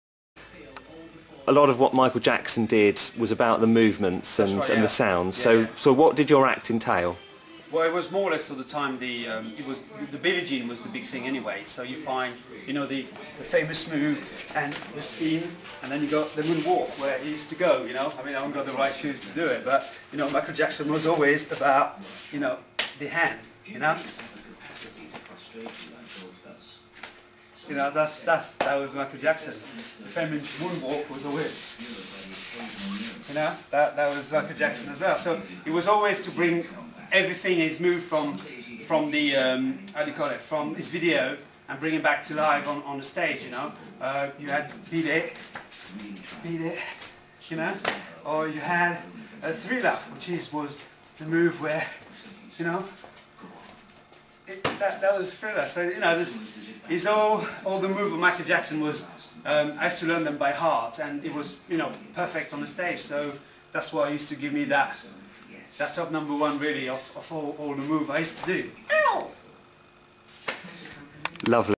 Oxfordshire Michael Jackson impersonator
Jean-Marc Bonere from Didcot shows Heart his moves - he used to be the No.1 Michael Jackson impersonator in Europe